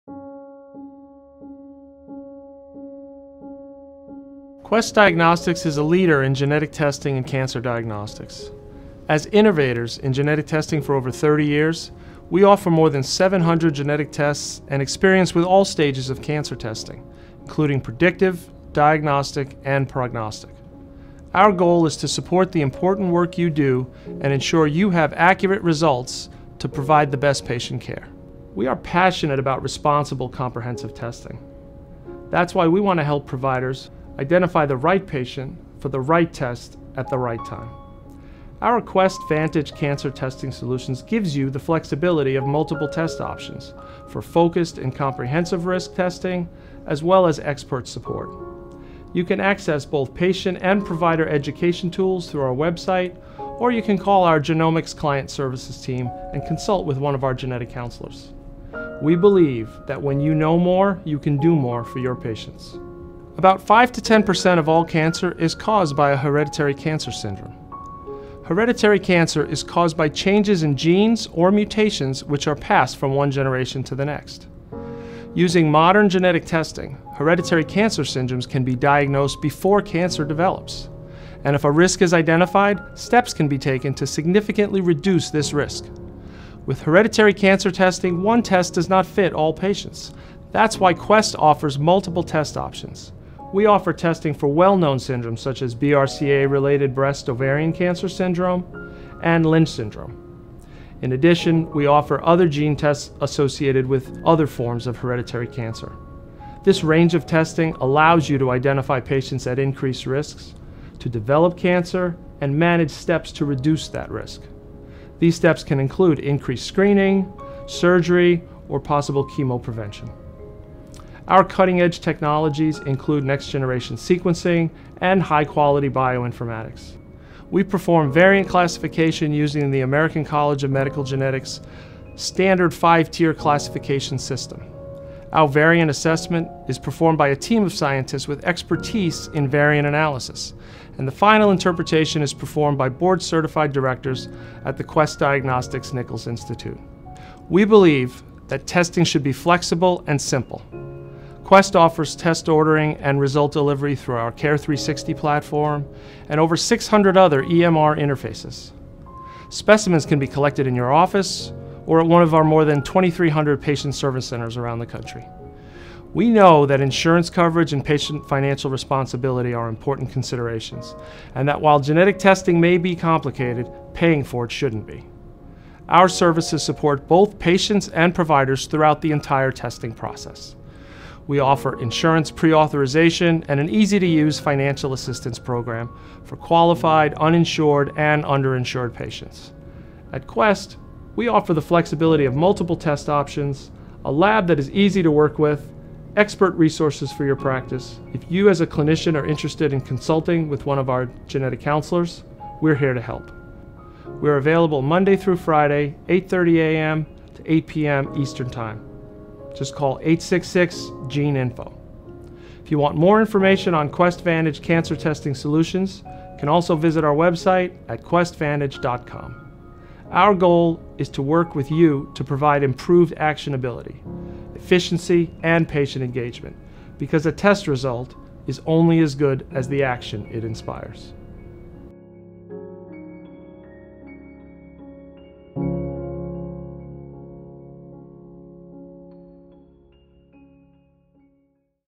Steve Keiles discusses QuestVantageᵀᴹ Hereditary Cancer Testing Solutions
Steve Keiles, Director of the Genetic Counselor Organization, discusses QuestVantageᵀᴹ Hereditary Cancer Testing Solutions